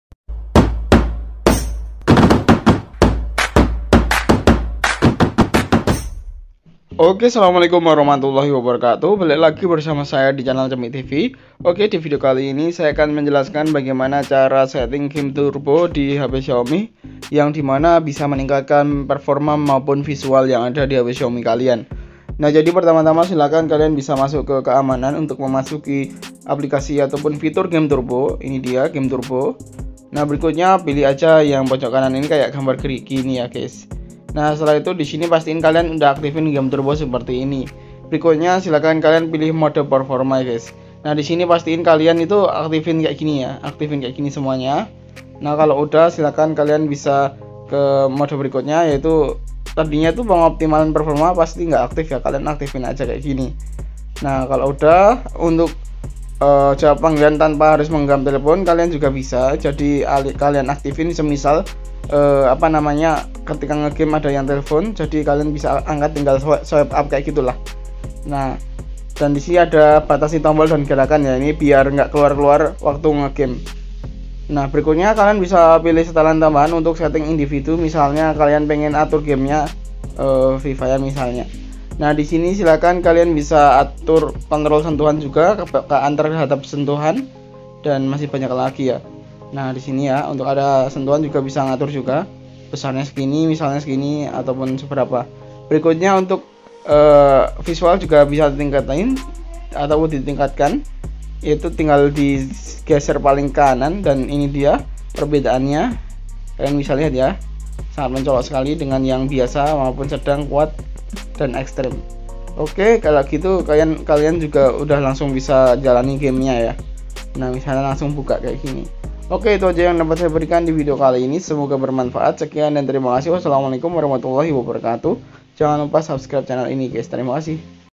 Cara Setting Game Turbo Xiaomi Biar Lebih Optimal Saat Main Game
pada video kali ini saya akan menjelaskan bagaimana cara setting game Turbo hp Xiaomi. semoga bermanfaat